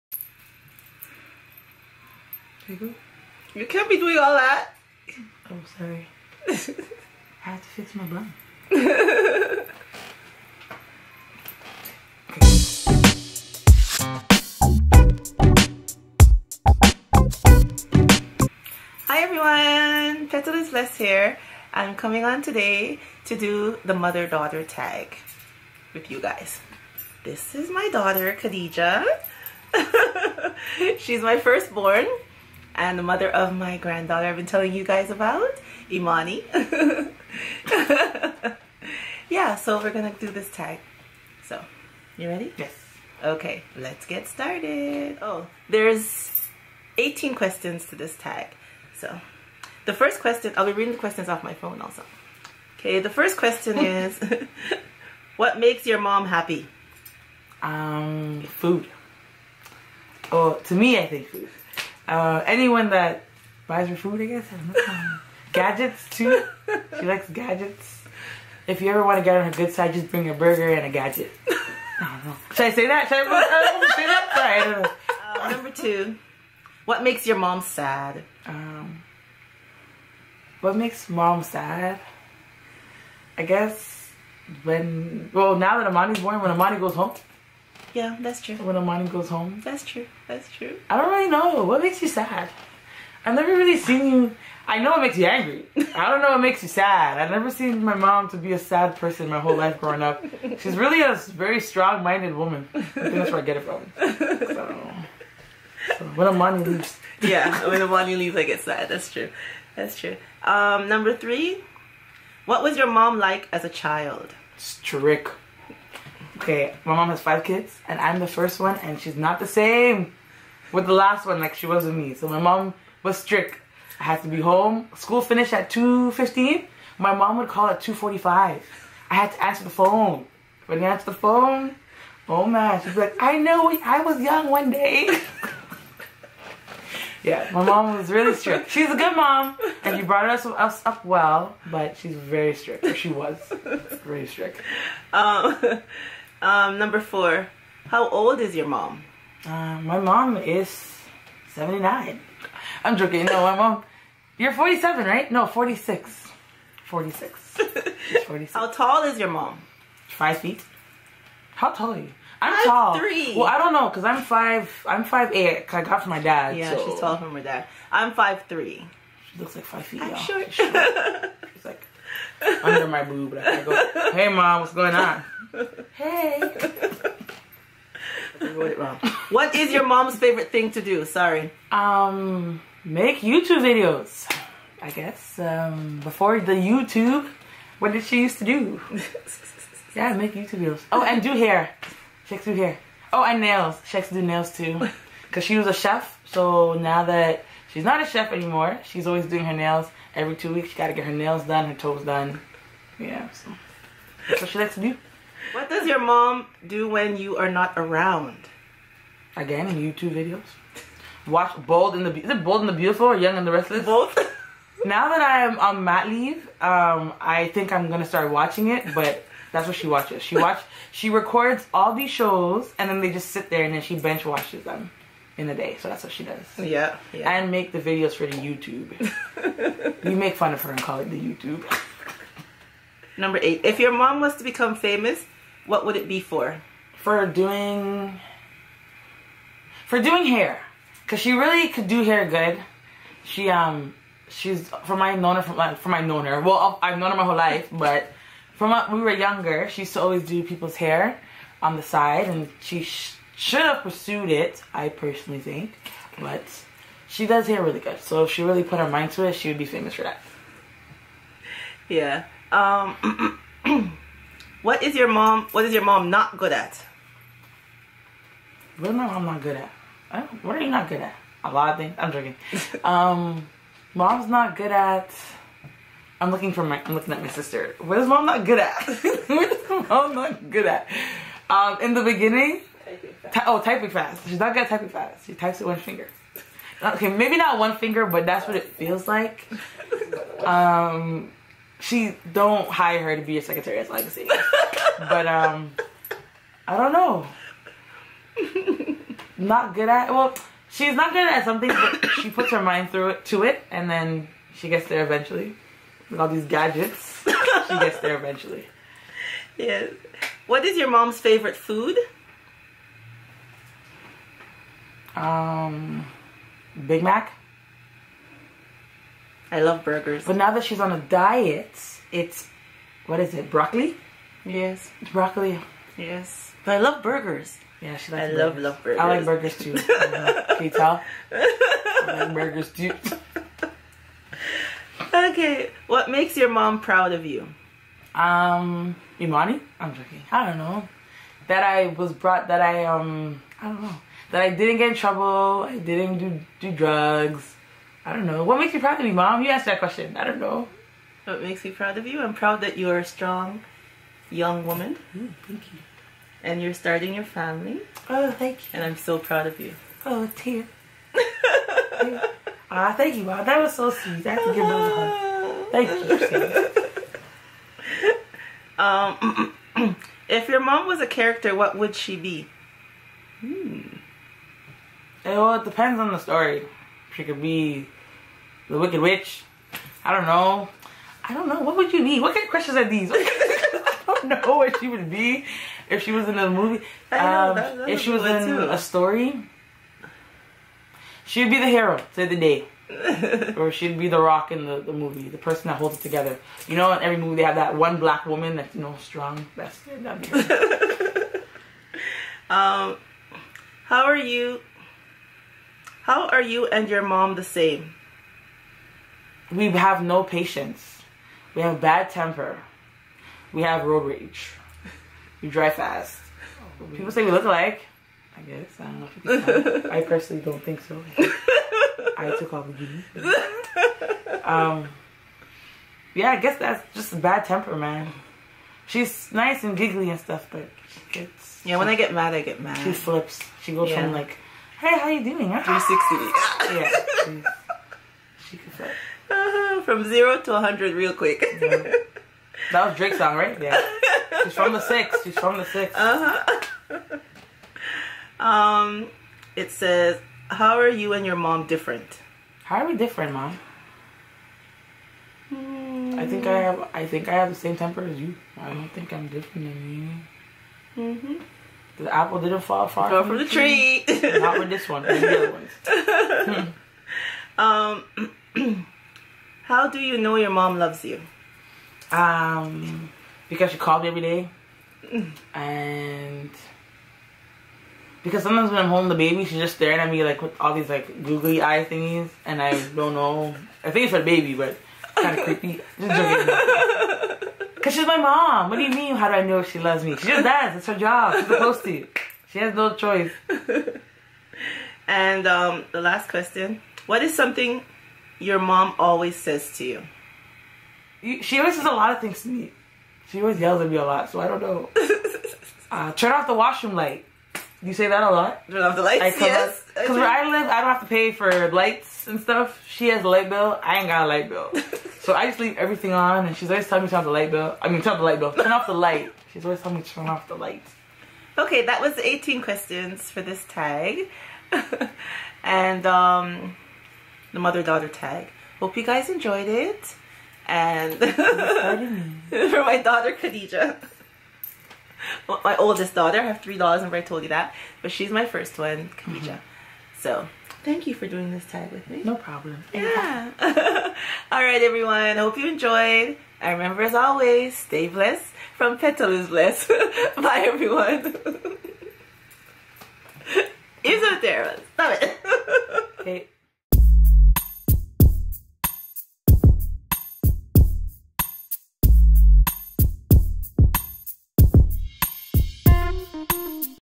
MOTHER DAUGHTER TAG 2016 | PETALISBLESS
Hey everyone I'm doing the Mother Daughter Tag 2016 featuring my oldest daughter. :)